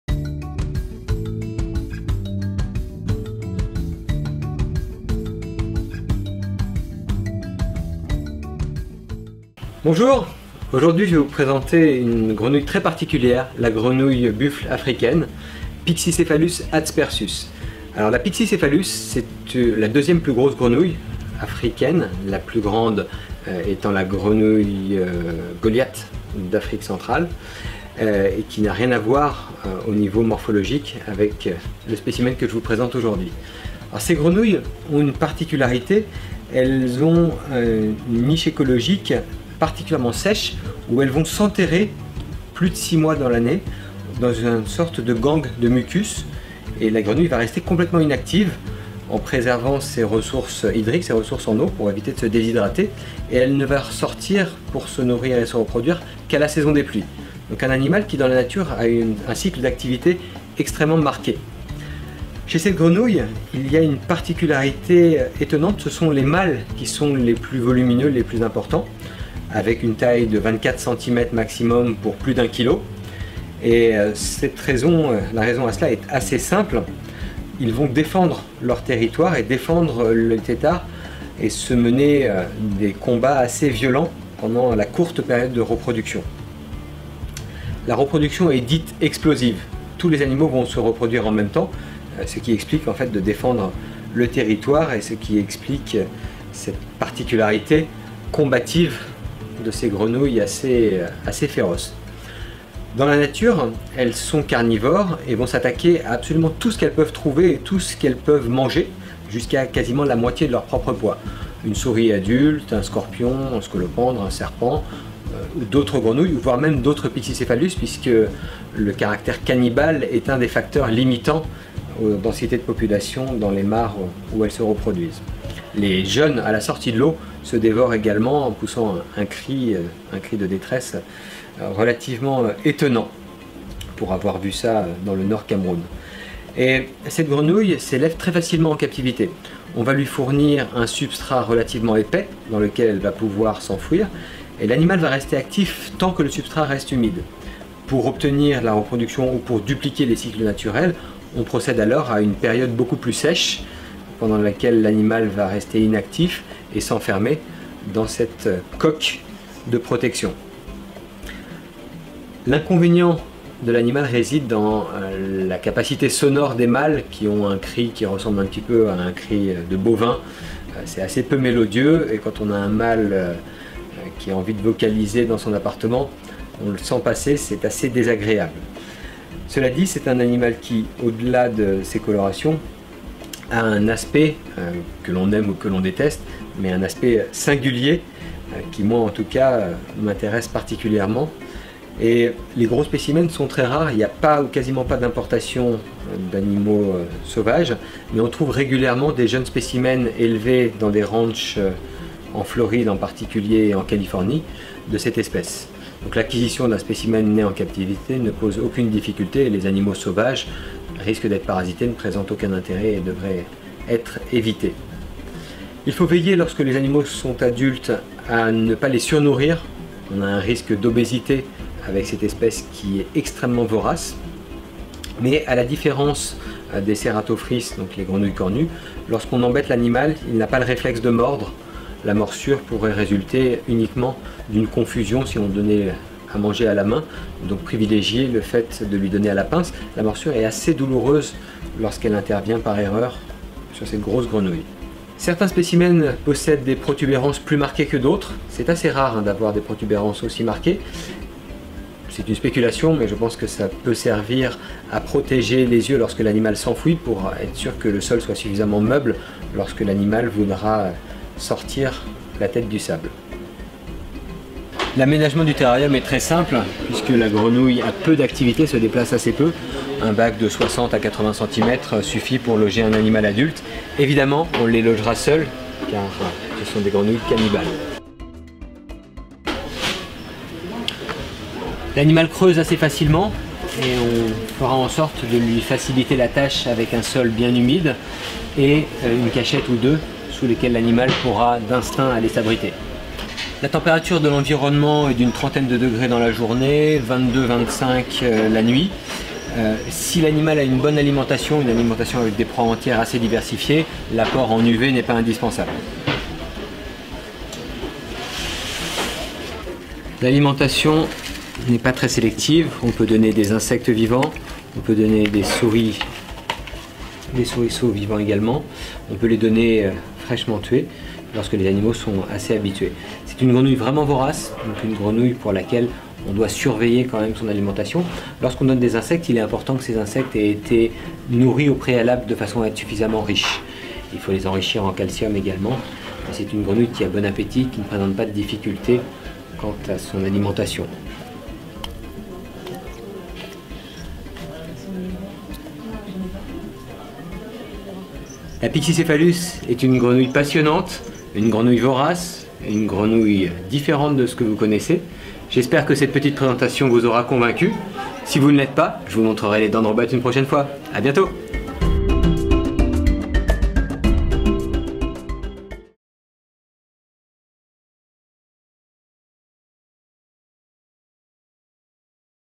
Les chroniques de La Ferme Tropicale - Ép. 2 - La grenouille taureau
Dans ce nouvel épisode, découvrez une espèce étonnante qui risque de bousculer vos préjugés sur les grenouilles : la Pyxicephalus adspersus ou grenouille taureau !